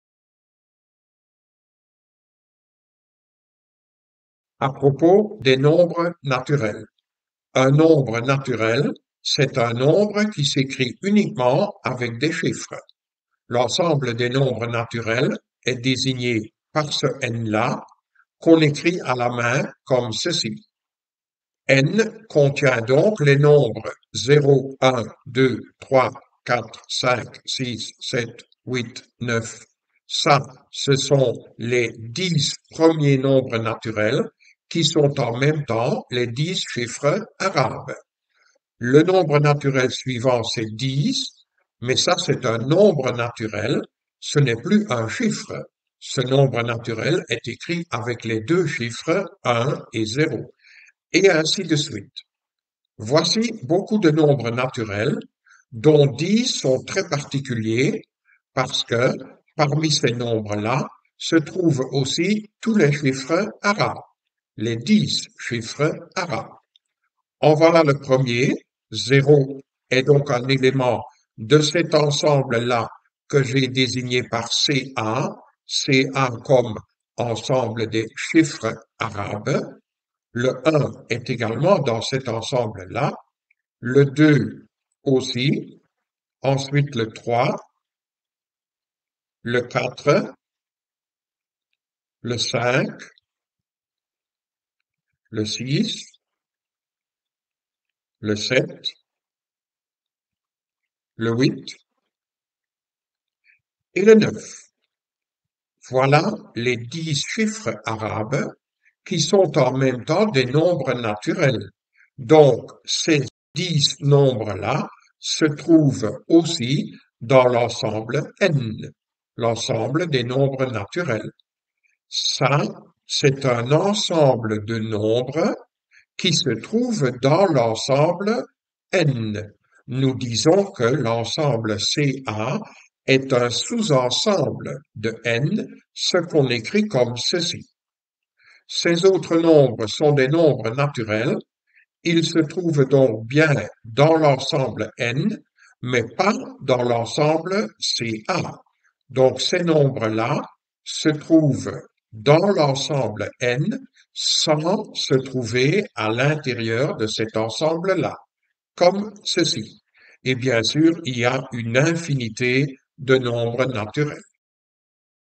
F1-2 002 Les nombres naturels
Quelle est la différence entre un chiffre et un nombre? Que signifie: est sous-ensemble de?